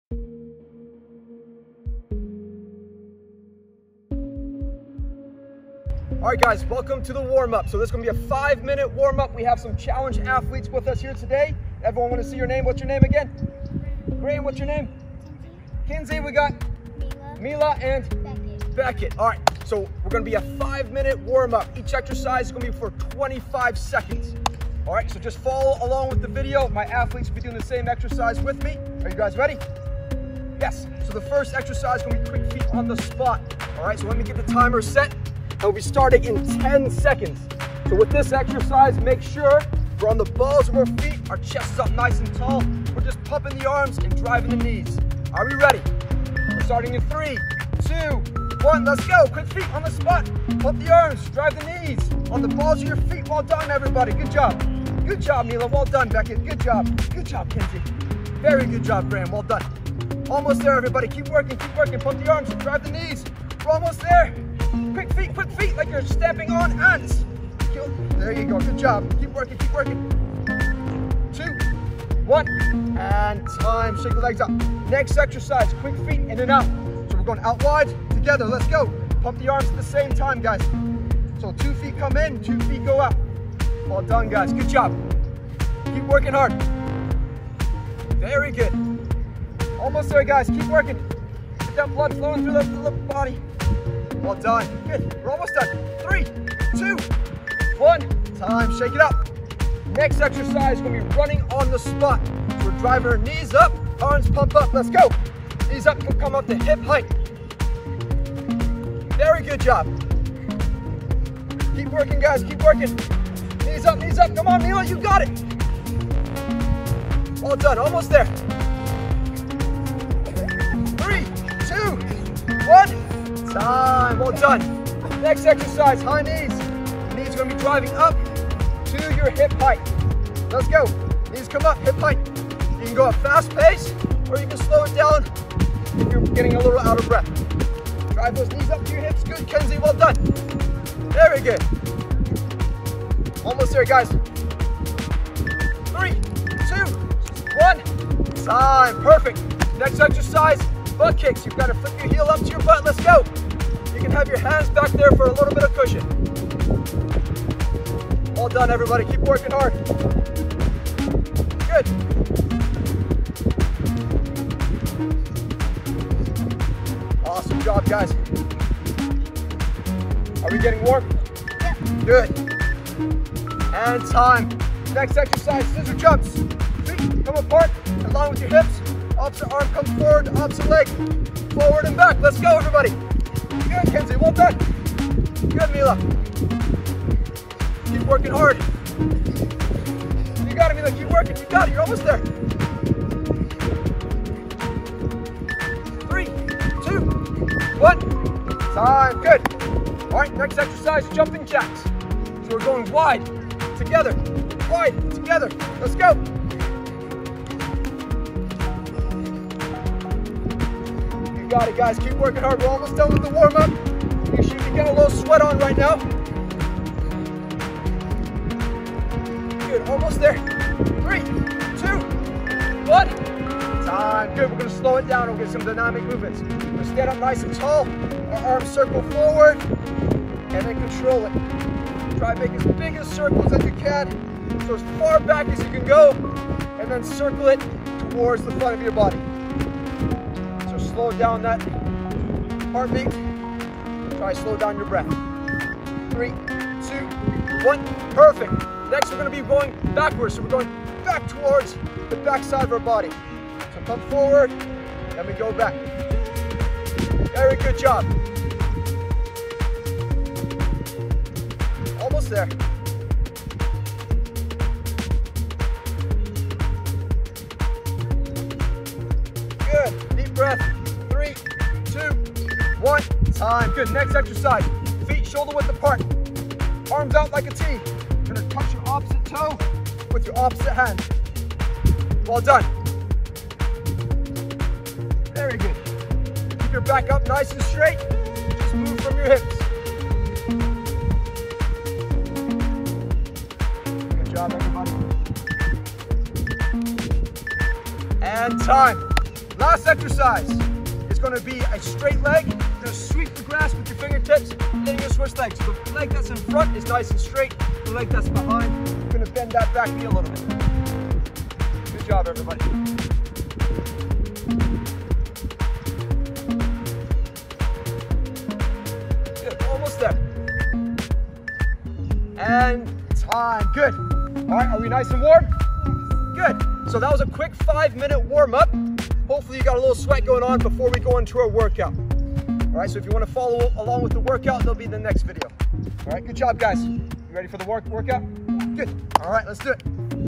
5 Min Warm up FOR KIDS
Warm Up:
(25 seconds per exercise)
• Quick feet on the spot
• Quick feet in and out
• Running on the spot
• High Knees
• Butt Kicks
• Scissor jumps
• Jumping Jacks
• Arm circles forward
• Arm circles backwards
• Opposite hand to toe touches (Windmill)
• Straight leg sweeps